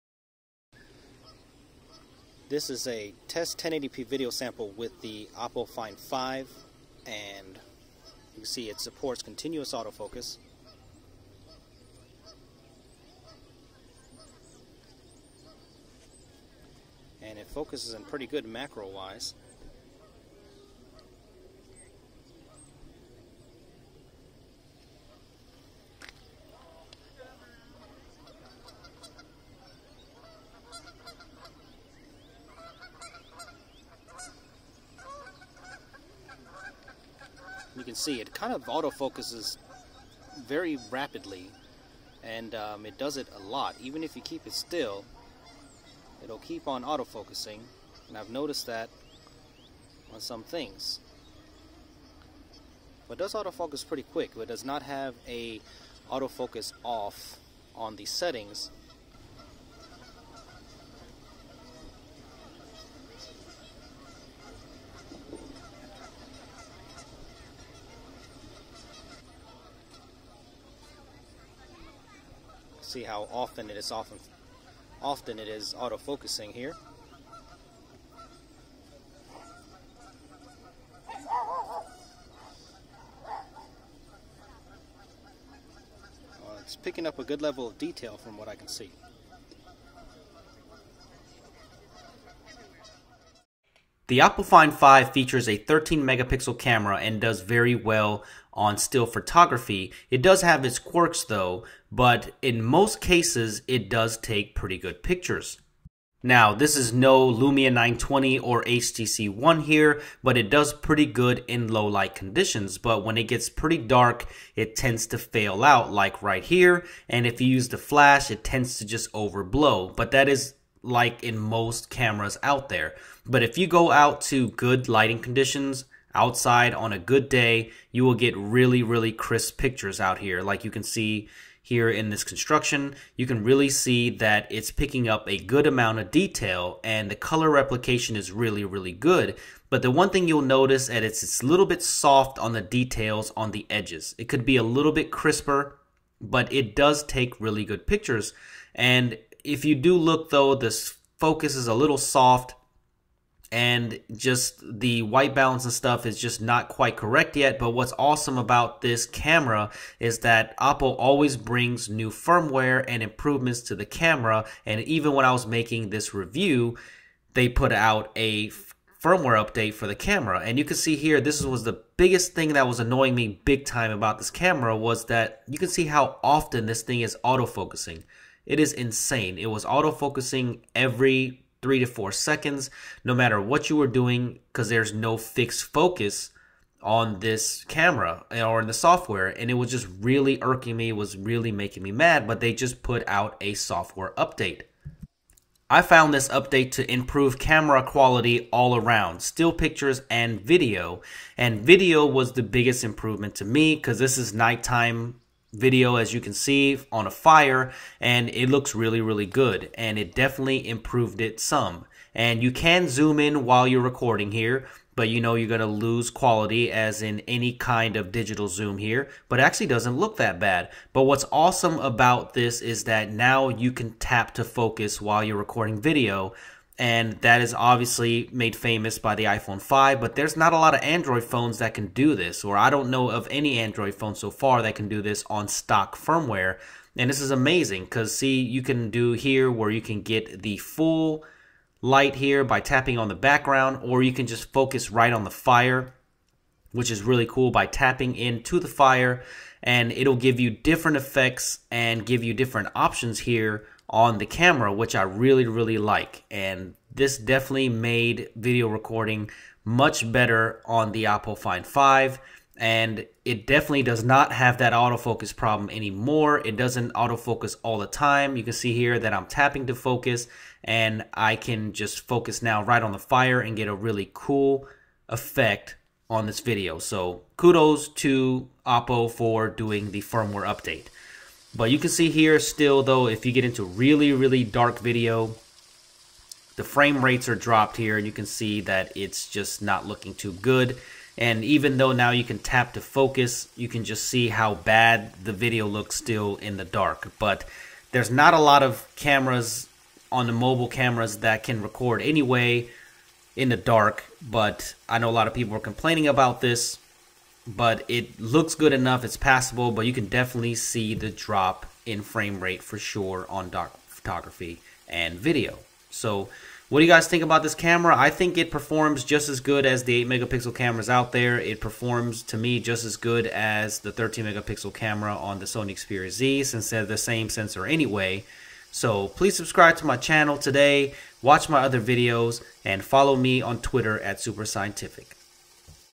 Oppo Find 5 Camera Samples (1080P HDR Video and Photos)
Watch as I test the 13MP camera on the Oppo Find 5.  The camera is capable of shooting HDR 1080P Video live.